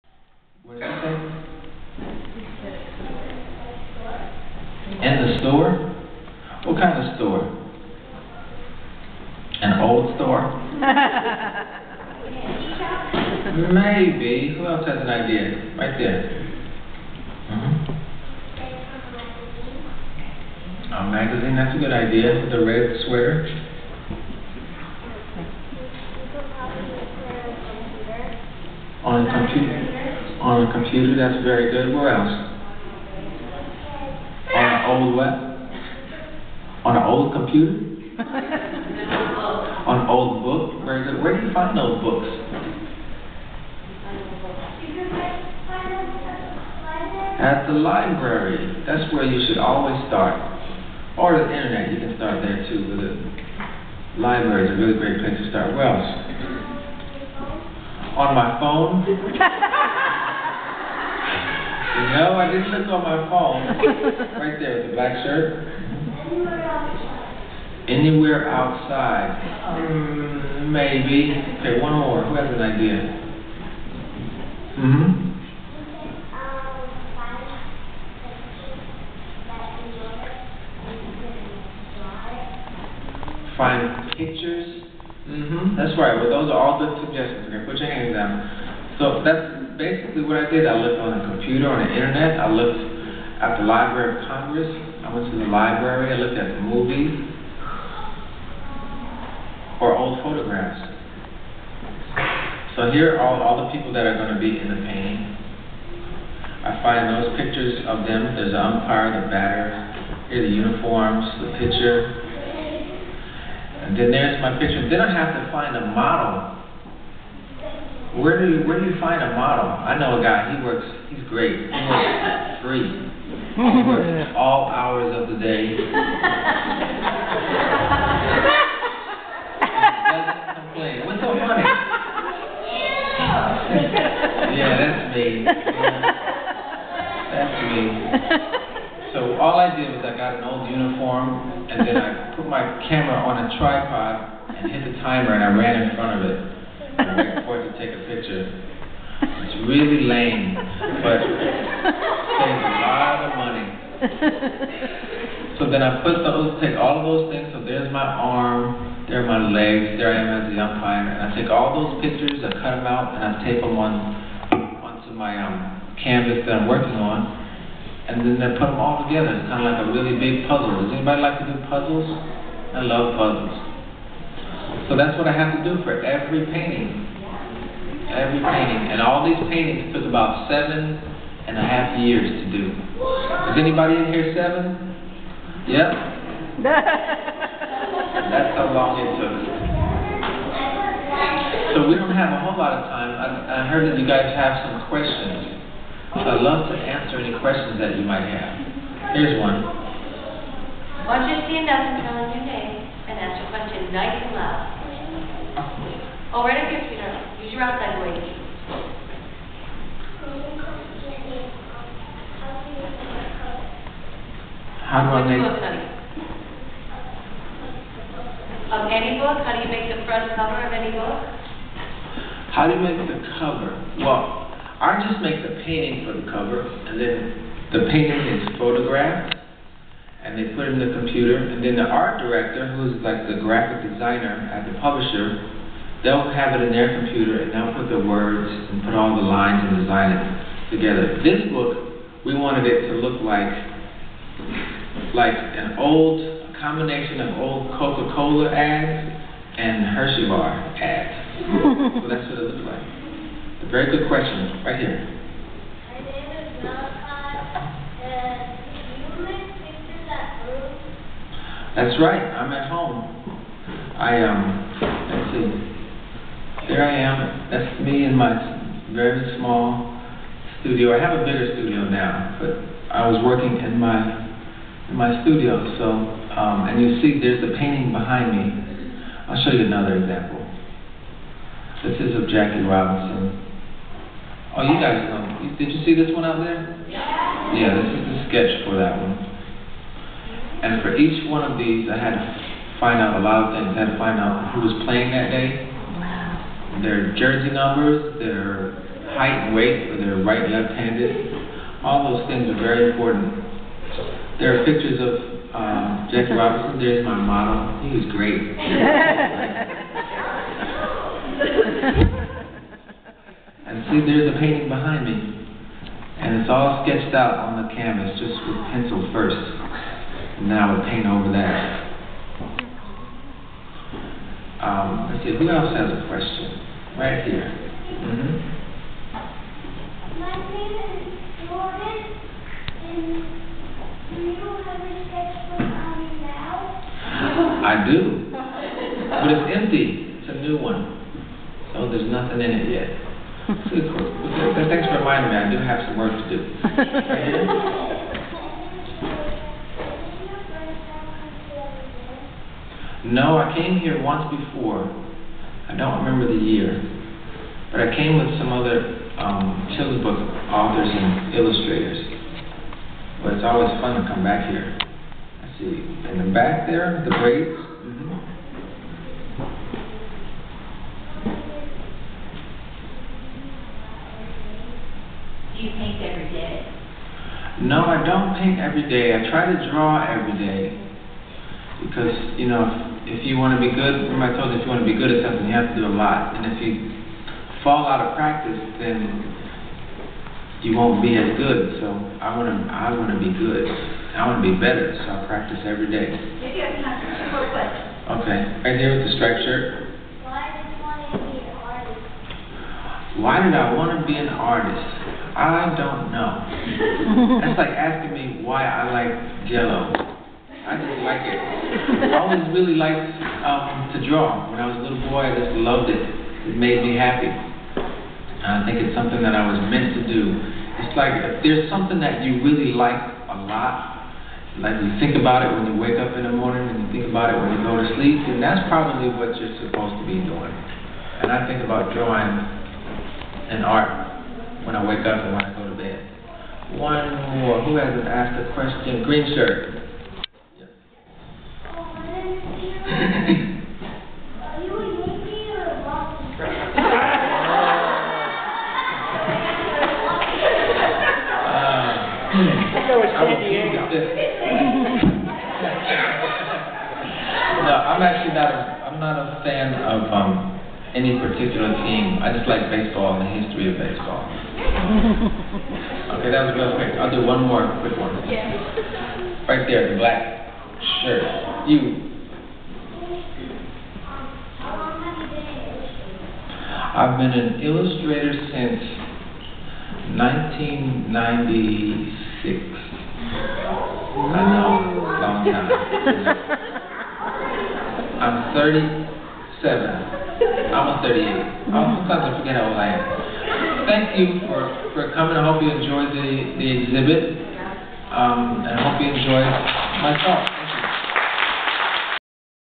Kadir Nelson at The Eric Carle Museum
Kadir Nelson answers how he researches his books at an appearance at The Eric Carle Museum of Picture Book Art.